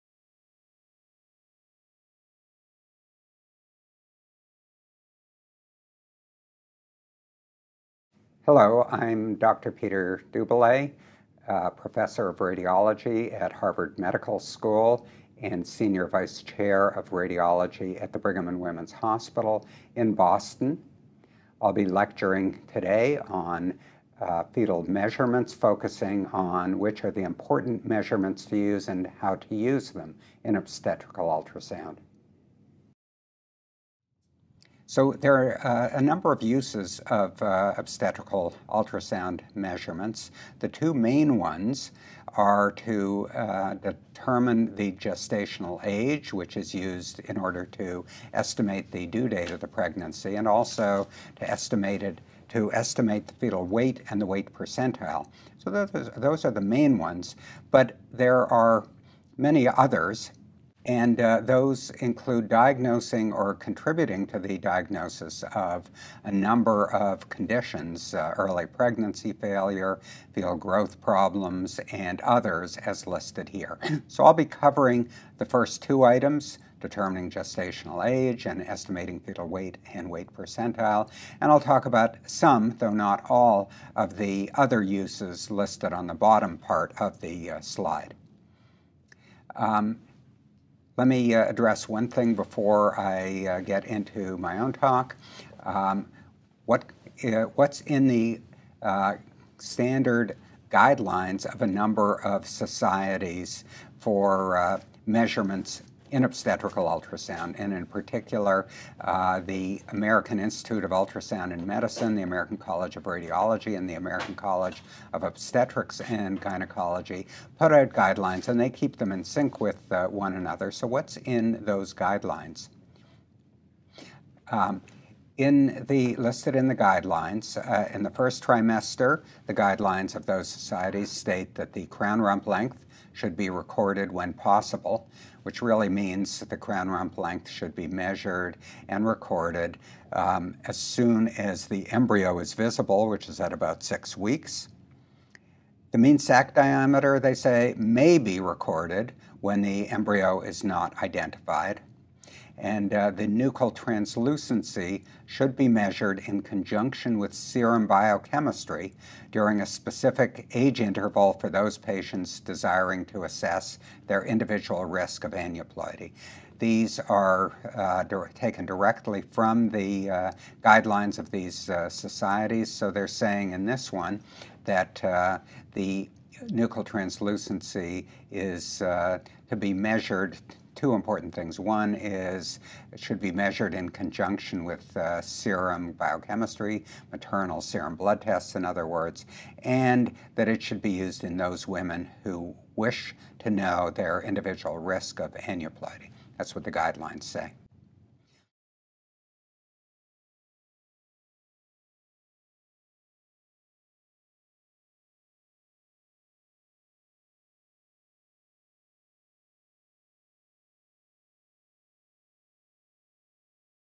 "Obstetrical Measurements: Which Ones are Important?" Ultrasound SonoWorld Lecture
Dr. Peter M. Doubilet presents an overview of the measurements used in obstetrical ultrasound and offers his professional opinion on those considered most important.

Upon completion of this educational activity the participant will be able to recognize the value of obstetrical measurements in assigning fetal age, diagnosing early pregnancy failure, intrauterine growth restriction, macrosomia and estimating fetal weight.